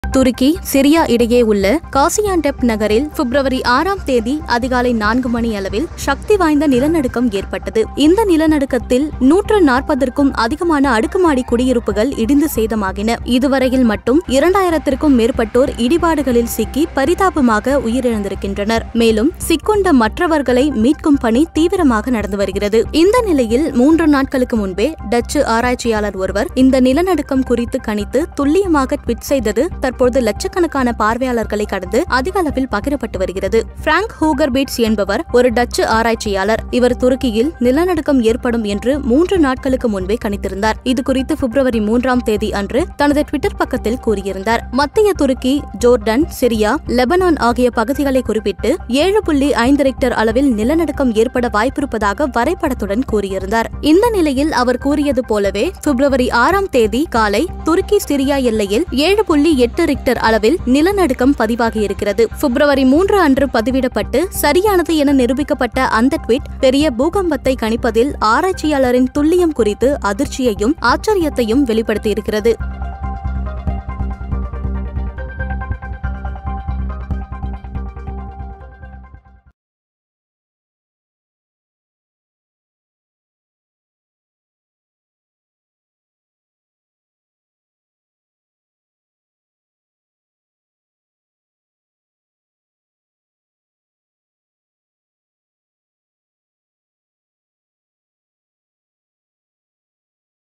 Turkey Earthquake : 3 நாள்களுக்கு முன்பே கணித்த டச்சு ஆராய்ச்சியாளர்? - வைரலாகும் ட்விட்டர் பதிவு!
Watch the latest updates on the 2023 Turkey earthquake. Footage captures the moment the earthquake struck and its aftermath, including damage to buildings and infrastructure. Our thoughts are with those affected by this tragedy.

எழுத்து - சு.மகாலெட்சுமி | ஒலிப்பதிவு - R.விக்னேஷ் | பின்னணி குரல் - Keerthiga.R | படத்தொகுப்பு - சிரஞ்சீவி